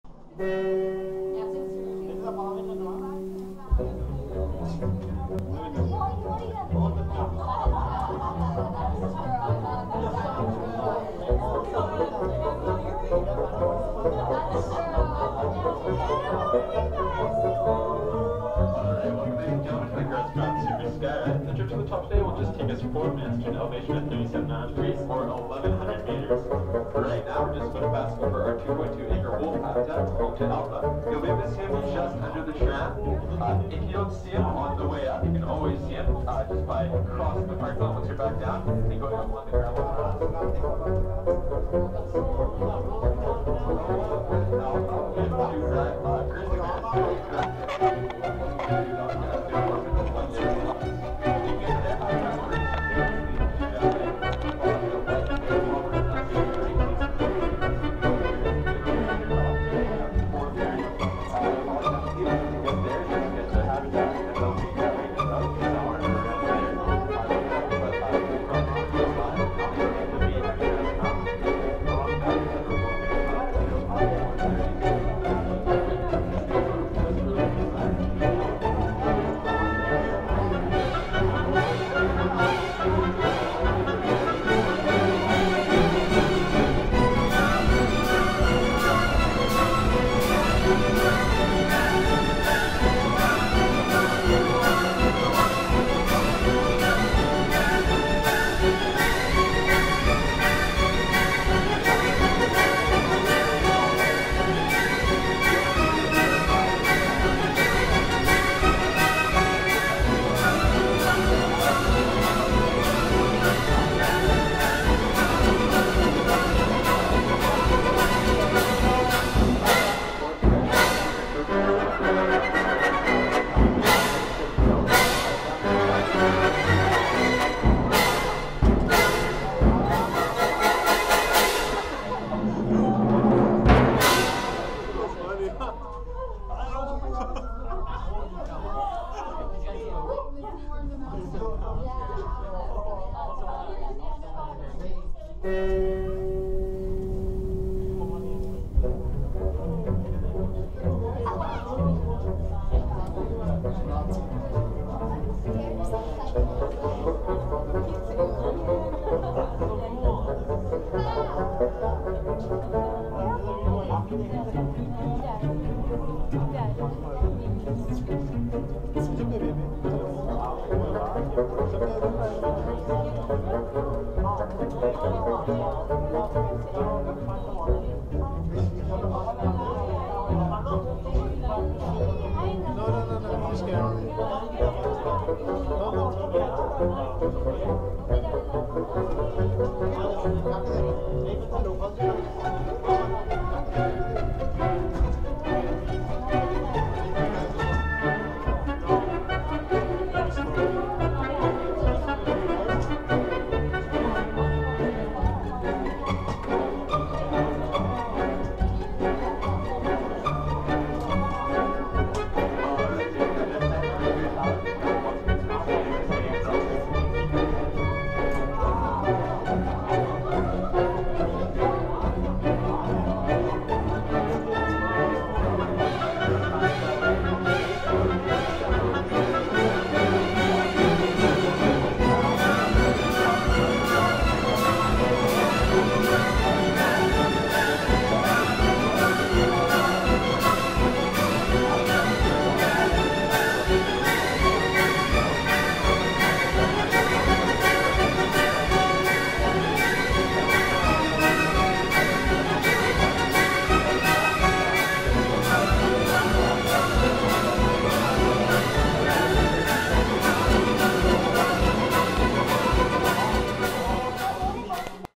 Sky Ride Up Grouse Mountain
Cable car ride up Grouse Mountain at Victoria BC Canada. via YouTube Capture